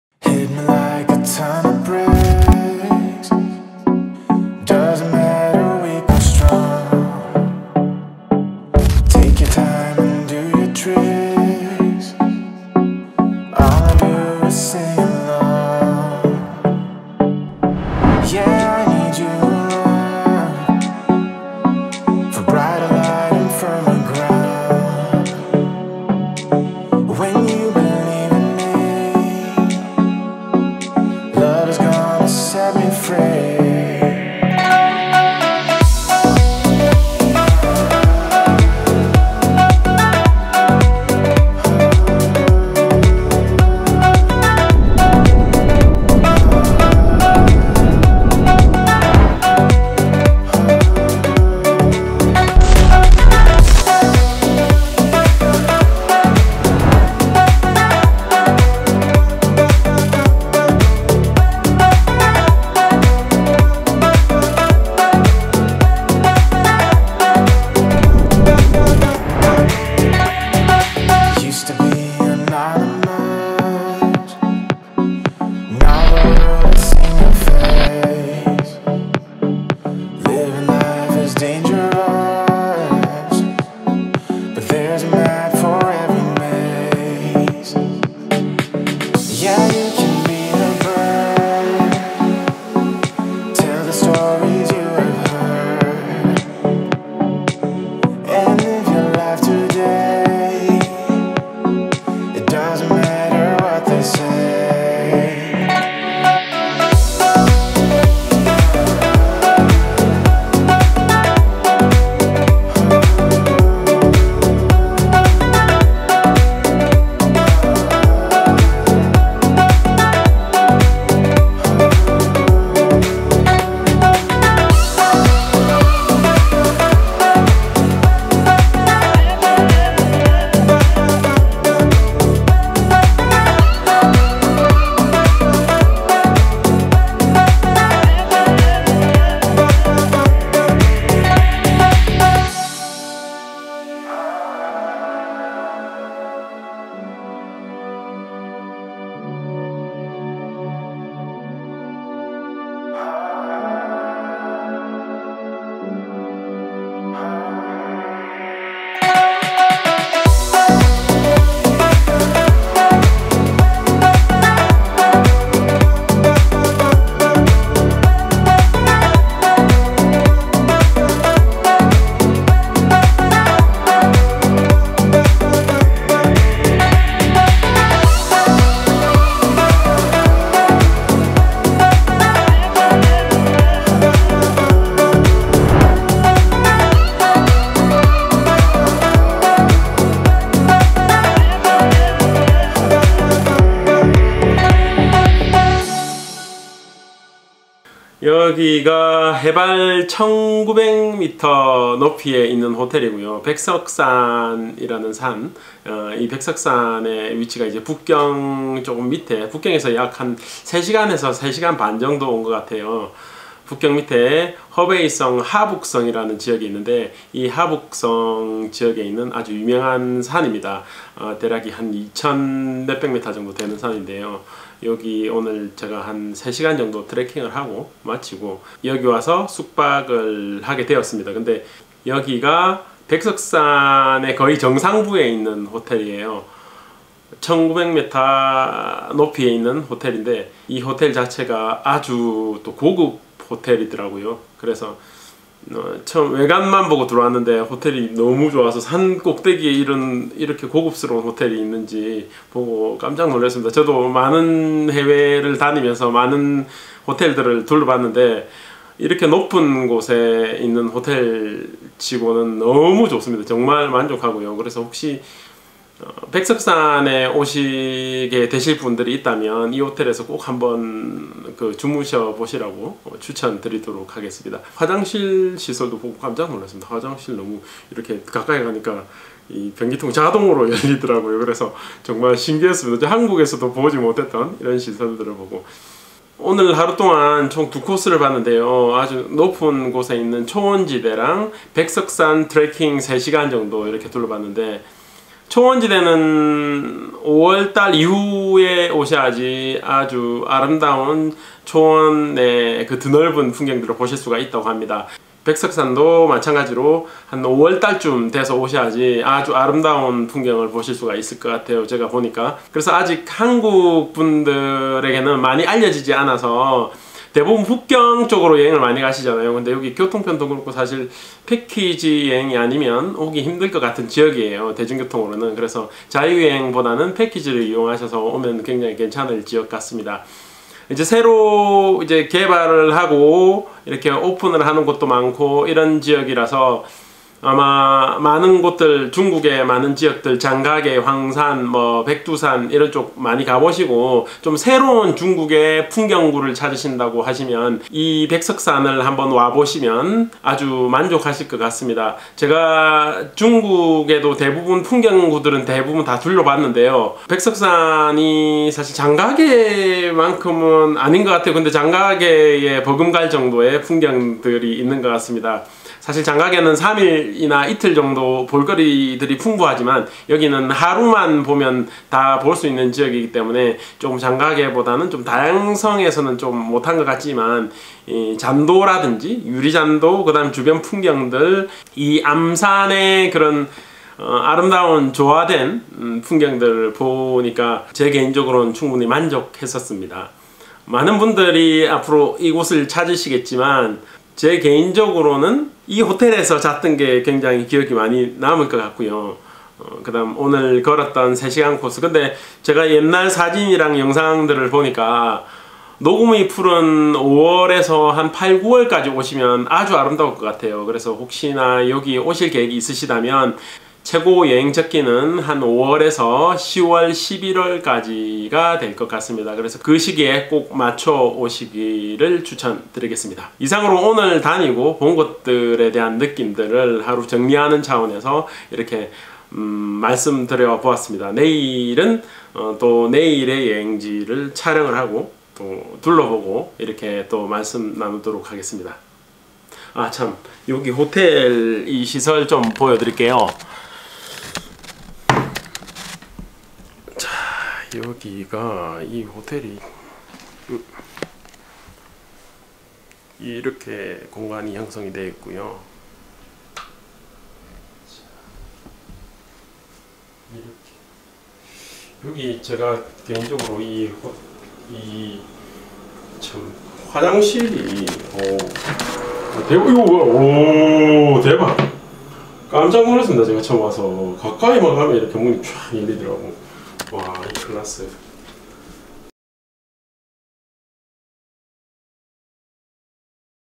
[Sketch] 중국 10대 명산 백석산 여행 / a trip to baishishan in china
이번 영상은 중국의 10대 절경 여행지로 손 꼽히는 백석산의 풍경들을 스케치 한 영상입니다.

먼저 이번에 답사여행으로 중국을 4일간 다녀왔고요. 북경 아래 허베이성과 북경 근교의 몇곳들을 둘러보고 왔습니다. 중국을 여러번 가보았지만 이번에 갔던 코스들은 모두 처음 가 보는 지역이었는데요. 특히 처음 가 보았던 백석산이 정말 인상 깊었습니다.

중국의 장가계, 계림, 황산, 백두산, 북경, 상해 모두 다녀오셨다면, 이제 백석산으로 한번 가 보시길 추천 드립니다. 아직은 잘 알려지지 않았지만, 아마도 새롭게 뜨는 중국의 뜨거운 관광지가 될걸로 예상됩니다. 그만큼 만족스러운 여행이었습니다.

10여일 이상 해외 여행중이었던 사정 때문에 지난 주에는 영상을 하나도 올리 지 못하여 2주만에 올리는 영상이 되어 버렸습니다. 백석산 영상 역시 이번 여행에 담아온 따끈따끈한 영상입니다. ^^

10여일간 3개국을 다녀 왔는데, 그 영상들 또한 빠르게 편집하여 올리도록 하겠습니다. ^^

오늘도 역시나 긴 영상 봐주셔서 감사드립니다.